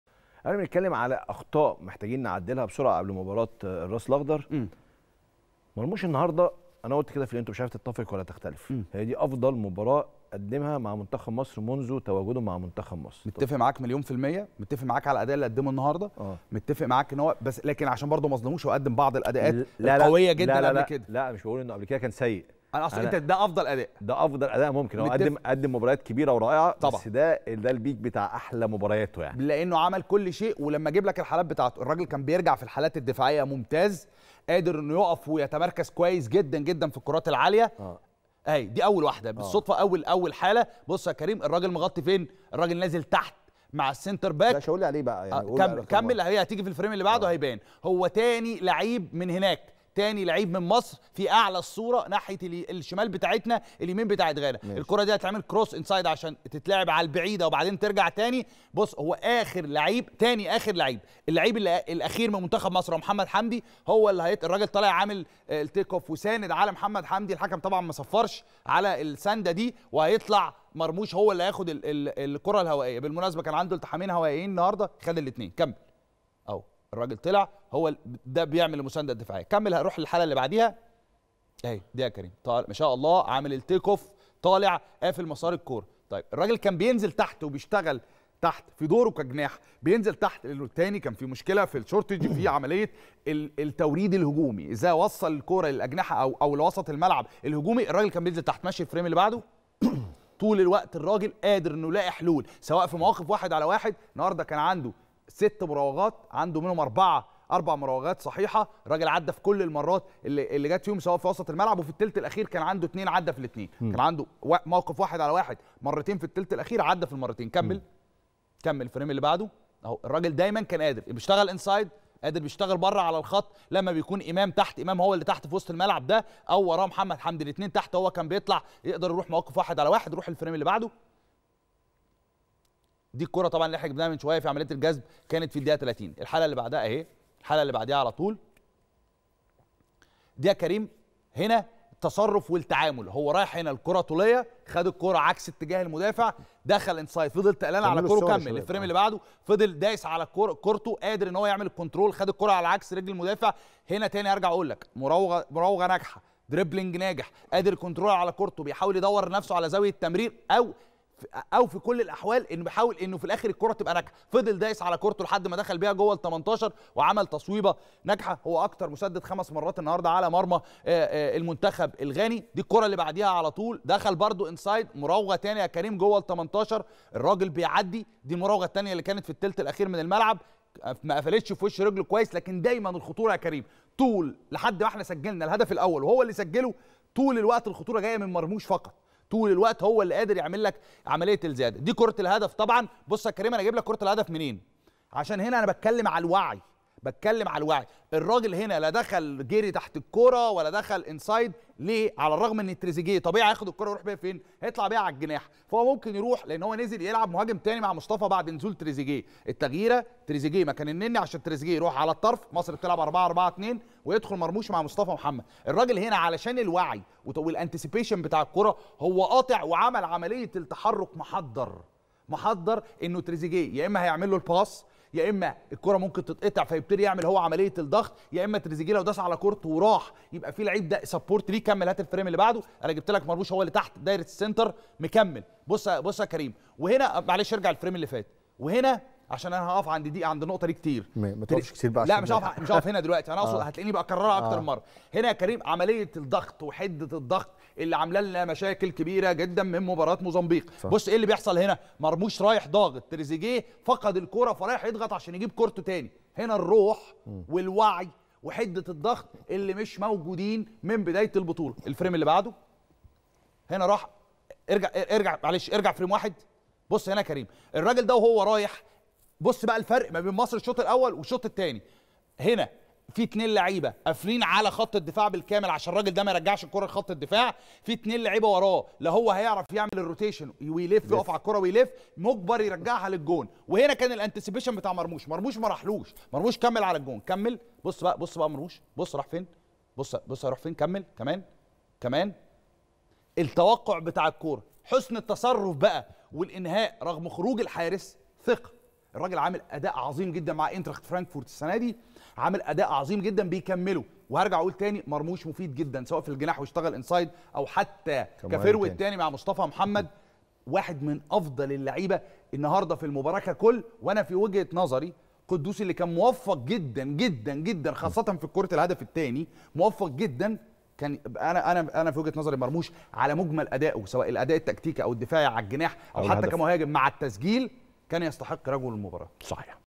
أفضل مبارياته مع المنتخب محمد جمال هيقولك باللقطات ليه مرموش كان الافضل ولقطة مجبتهاش الكاميرات
"أفضل مبارياته مع المنتخب" 💪🏼🔥محمد جمال هيقولك باللقطات ليه مرموش كان الافضل ولقطة مجبتهاش الكاميرات في لقطة الهدف👏🏻